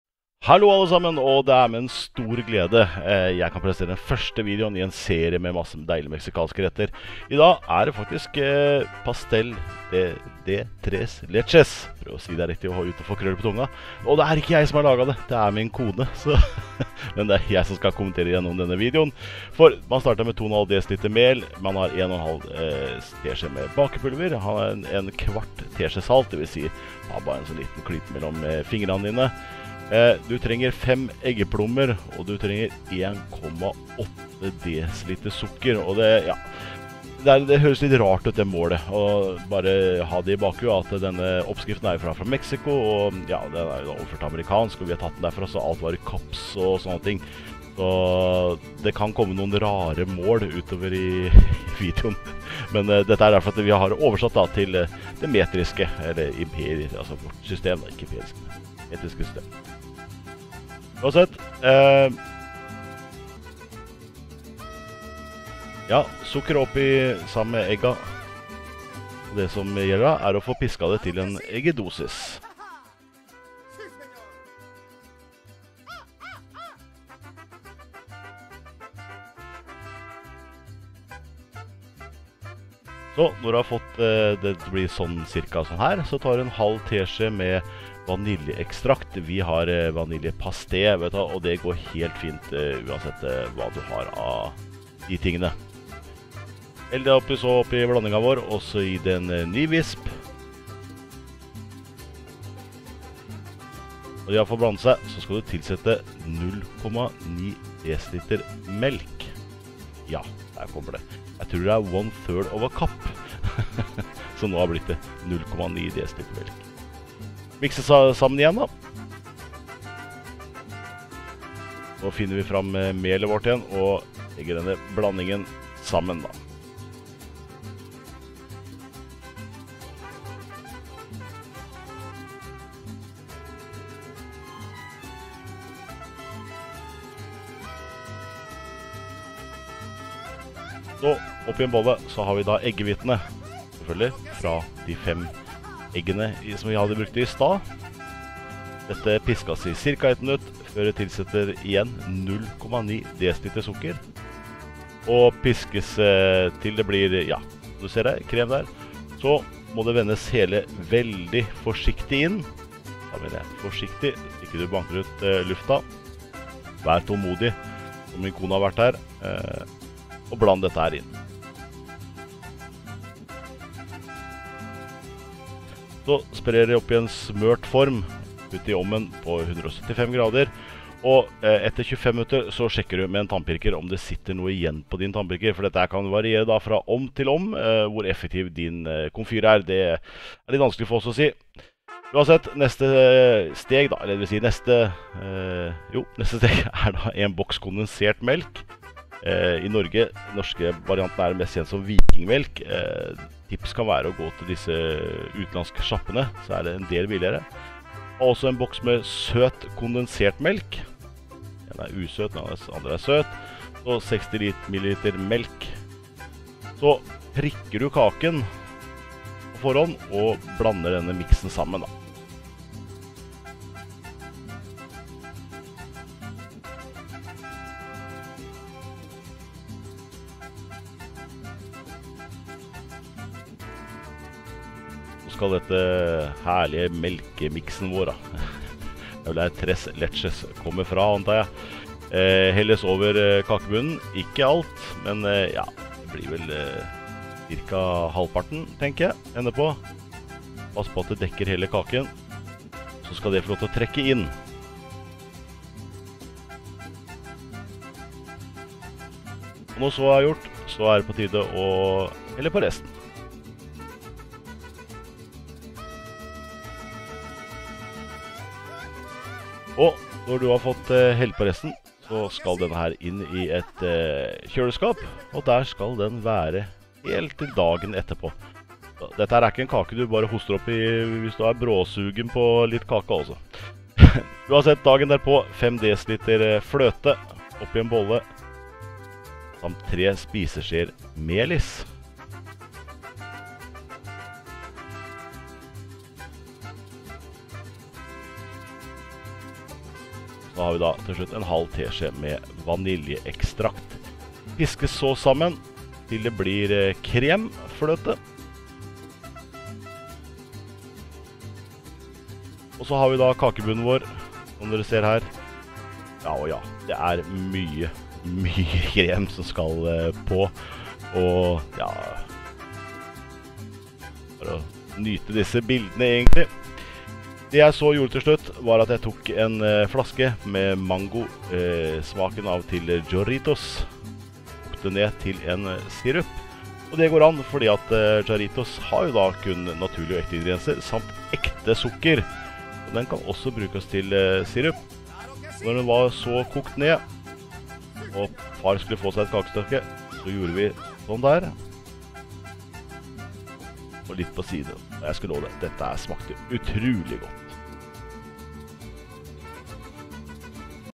Pastel De Tres Leches med Sirup fra Jarritos Mango
Tres leches
2,5 dl mel
1 1\2 ts bakepulver
1\4 ts salt
5 eggeplommer
1,8 dl sukker

Piske til eggedosis

1\2 ts Vaniljepaste eller ekstrakt
0,9 dl melk

Bland sammen med melet

5 eggehvitter piskes i cirka 1 minutt.
Så tilsettes 0,9 dl sukker og piskes igjen

Vendes så forsiktig inn

175 grader 25 minutter

1 boks kondensert melk
1 boks med søt kondensert melk
60 ml melk

5 Dl fløte
3 SS Melis
1\2 ts vaniljeekstrakt

Ønsker du å støtte kanalen for flere, bedre og spennende
mat-videoer, doner hvis du ønsker her:
Tusen hjertelig takk for støtten.